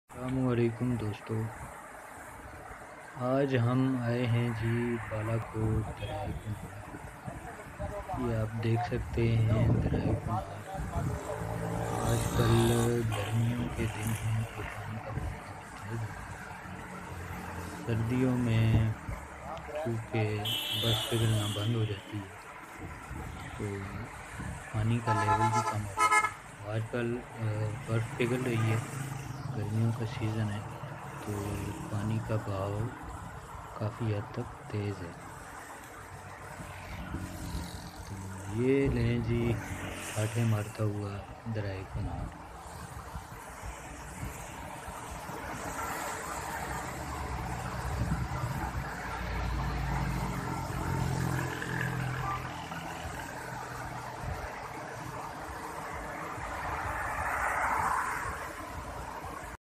Kunhar River
The Kunhar River (Urdu: دریائے کنہار) or River Kunhar, is a 166 kilometres (103 mi) long river, located primarily in the Khyber Pakhtunkhwa province, northern Pakistan. It is in the Indus River watershed basin.

Its previous name was NainSukh (Ankhon ka Araam). Some people claim after the arrival of Swatis in Hazara, this river was renamed because Swatis were originally from Kunar, Province of Afghanistan.